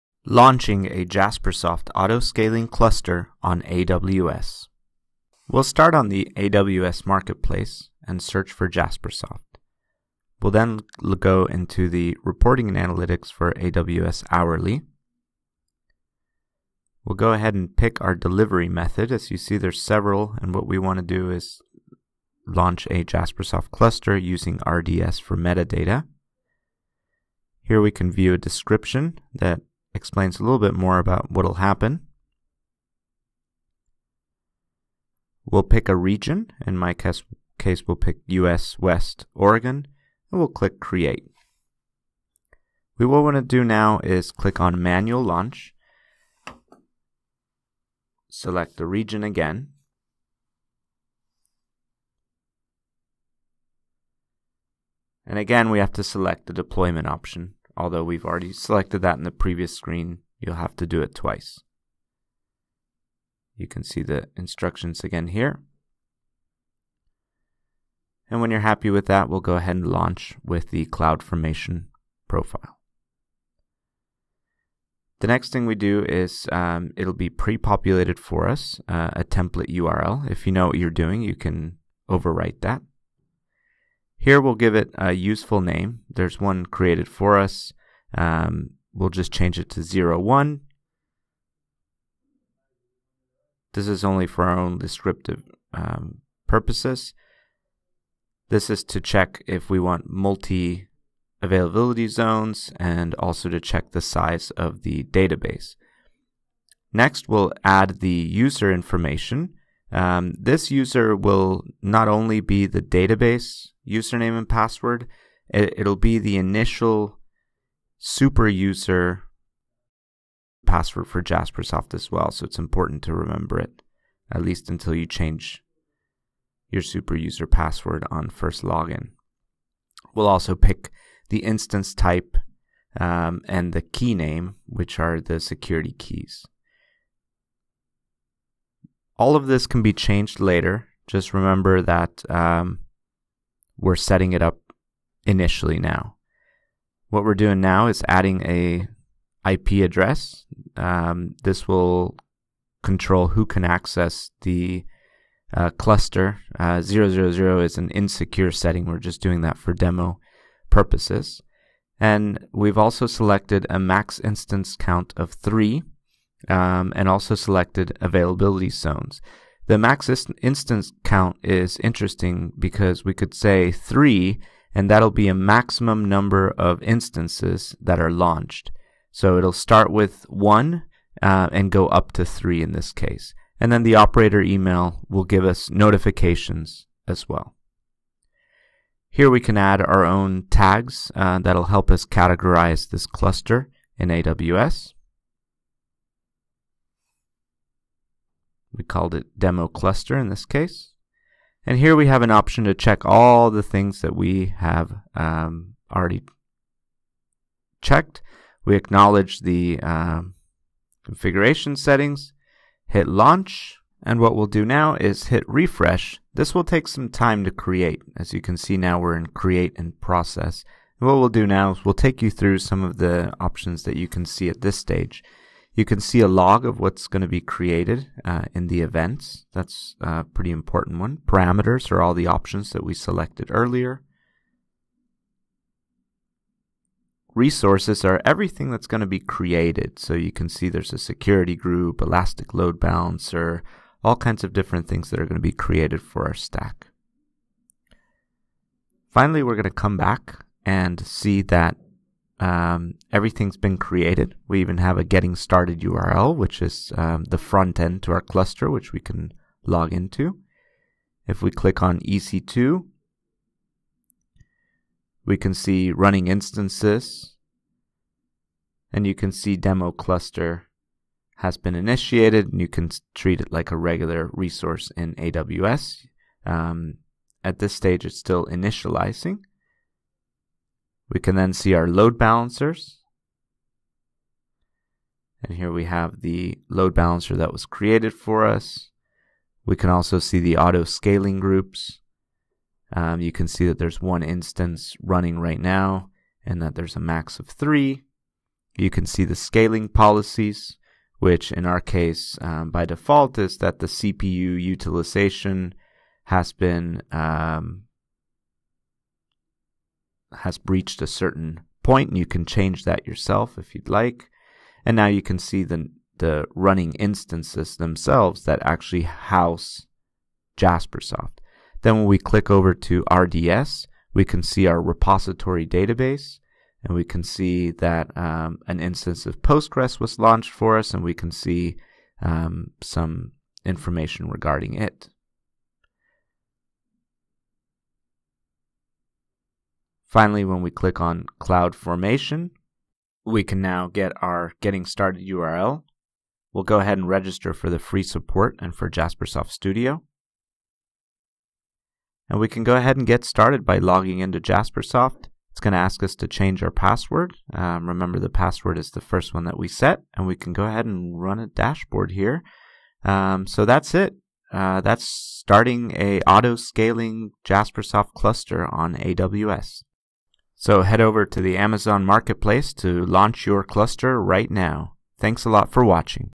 TIBCO Jaspersoft for AWS - Auto Scaling Clusters (Full Demo)
Learn about creating auto scaling, highly available infrastructure for your TIBCO Jaspersoft Stack. The JasperReports Server metadata repository DB is launched in a separate RDS instance and gives you the option to enable an ELB (Elastic Load Balancer) with an auto scaling group in front of your JasperReports Server instances to provide a static endpoint (DNS Name/URL) for your cluster. The automatic high availability setup creates an RDS DB for the JasperReports Server metadata repository configured with RDS failover and cache invalidation to help migrate from unresponsive nodes. This includes defining the AMI to be used for your new EC2 instance, an ELB, an autoscaling group with the max and minimum number of instance, an RDS Postgres instance, an IAM Role to set the EC2 credentials, and will define policies to allow this role to auto-discover and connect to AWS data sources.